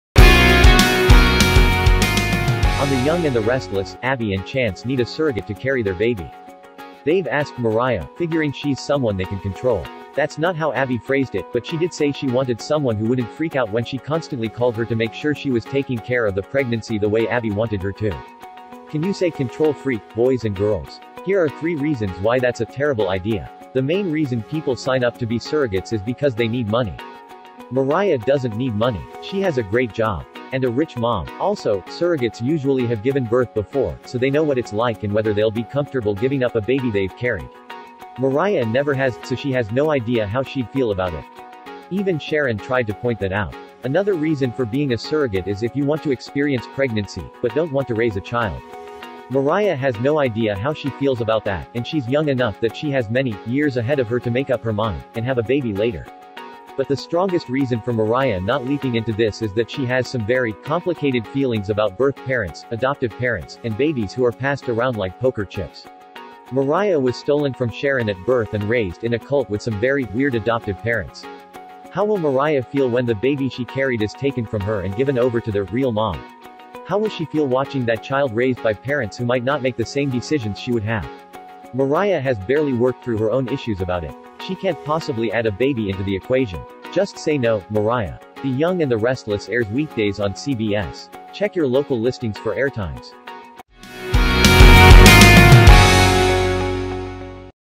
Why Mariah Shouldn't Be Abby's Surrogate On The Young and the Restless
On The Young and the Restless, Abby and Chance need a surrogate to carry their baby. They've asked Mariah, figuring she's someone they can control.